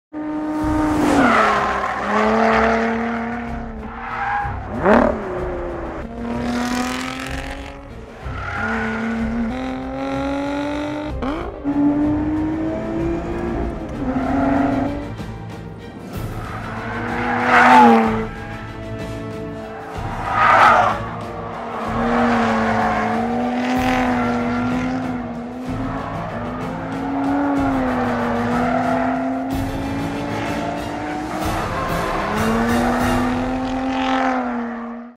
Lotus Evora 400 - Always True, Always Exciting, Always Original
There are cars and there are legends! Everyone has their dreams, but precious few make them happen.

On September 19, 2018, Lotus opened a new 5000 square feet showroom in Dubai, capable of showcasing up to 10 cars.

On display at the inaugural event was the limited-edition Lotus Exige Cup 430 Type 25 with 3 of the worldwide volume of 25 units making their way to the GCC; the only Lotus 3-Eleven 430 in the Middle East finished in the famous Lotus black and gold livery; and the internationally acclaimed and fastest Lotus production vehicle ever, the Lotus Evora GT430.

Videos courtesy of Lotus Group